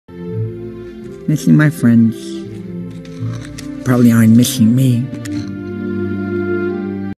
I'm forgotten.....
I was right. they did forget me. they must be happy now that they're better off without me. all I ever do was get on their nerves with my depression, that's why they forgot me...so they won't deal with me anymore. Iooks like I am alone now.